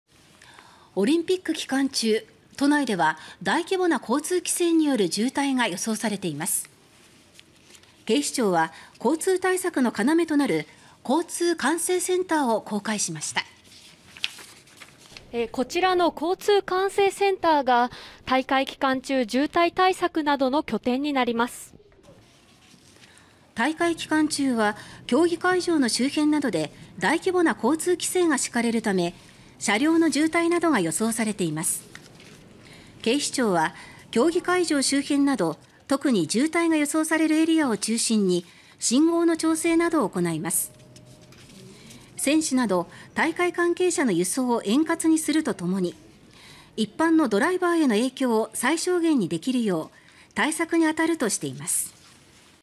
五輪期間中の渋滞などに対応 交通管制センター公開(2021年7月16日)
オリンピック期間中、都内では大規模な交通規制による渋滞が予想されています。警視庁は交通対策の要となる「交通管制センター」を公開しました。

　大会期間中は競技会場の周辺などで大規模な交通規制が敷かれるため、車両の渋滞などが予想されています。

　警視庁は競技会場周辺など、特に渋滞が予想されるエリアを中心に信号の調整などを行います。

　選手など大会関係者の輸送を円滑にするとともに、一般のドライバーへの影響を最小限にできるよう対策にあたるとしています。